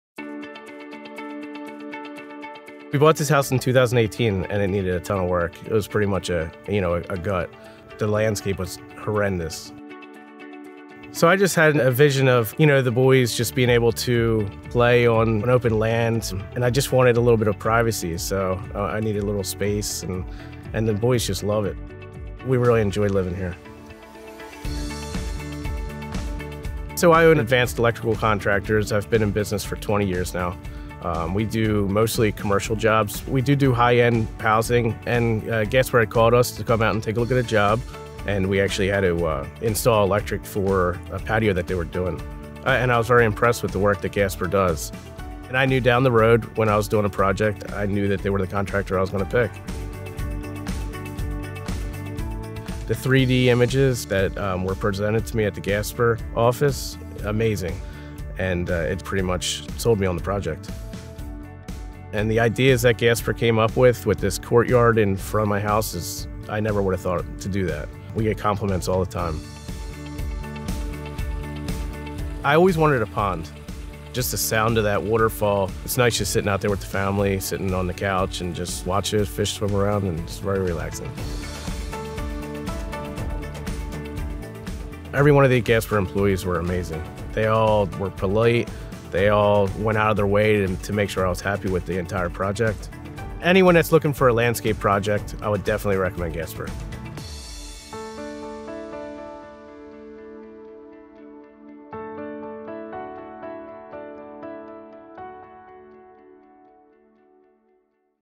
The Supercharged Yard
Gasper’s renovation of this front and rear yard for local Electrician and his family is truly electrifying!

Greg & his wife Christine moved to this property in 2018 with their four boys.  The 3.5 acre property had a lot of open space for the kid to play, but the house and landscaping around it was in need of a complete rehab.  They started with the house, taking it down to the studs, redoing the roof, replacing the windows and adding an extension.  They also did a lot of re-grading to level and reestablish the perfect lawn for the kids.

Greg and his wife own Advanced Electric Contractors which has been in business for over 20 years.  He mainly does large scale commercial electrical work, but he has collaborated with Gasper on a few residential projects (Shout out to the fabulous café lights he installed over his own patio!).  So when they were ready to tackle a new front yard and renovations to the deteriorating back patio, they reached out to Gasper.

Greg and his wife were blown away by Gasper’s initial proposal to create a front courtyard.  “It wasn’t anything I would have come up with”, remembers Greg, “and the 3D model, sold me on the job.     I loved seeing exactly what my project was going to look like.”  Instead of the old concrete sidewalk, Gasper installed new dry laid bluestone walks with a clay brick border and bluestone supertread steps.  At the center point of the new courtyard where the views of the front door and the new walkway meet, a circular landing with a large cast stone urn was designed.  The urn is planted with seasonal plants and is a visual focal point both from the house and porch and for guest arriving via the front walk.

In the back, there was an existing deteriorating brick patio with brick walls that were uncapped.  Gasper removed the brick patio and replaced it with a wet laid Pennsylvania bluestone patio with clay brick accents that matched the brick on the house perfectly.  The existing walls were repaired and repointed, then capped with bluestone, which not only gives the wall a more finished appearance, but also helps to protect the wall from water damage.  An old unusable exterior fireplace, a previously dead space, was transformed into a cooking station for the new gas grill, complete with granite countertops.

Greg had always wanted a pond and the Gasper designer was able to place the pond so that the family could overlook the pond year round from their sun room, where they usually eat dinner.  “I love to watch it from the dinner table,” says Greg. The sound of running water is particularly soothing and makes sitting around the fire table even more relaxing.

As an owner of a successful business, Greg recognized the quality of Gasper’s workmanship and its employees. He was very impressed that the Gasper crews stayed on the job the entire time.  There was no times where they pulled off the job.  Plus everyone from the Gasper team was incredibly polite and respectful.  “They went out of their way to make sure I was satisfied”, recalls Greg.   “We get tons of compliments on the work.”

Want to see how Gasper can electrify your outdoor space?